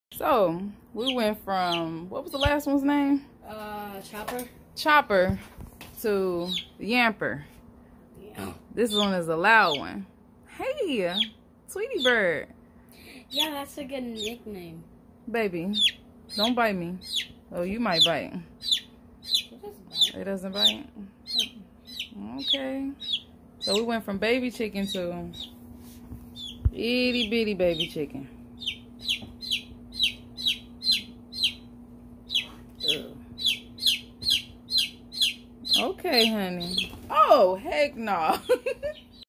Yamper The Chickadee
In the middle of the day I just said duck it let's go to the beach. lmao don't be like me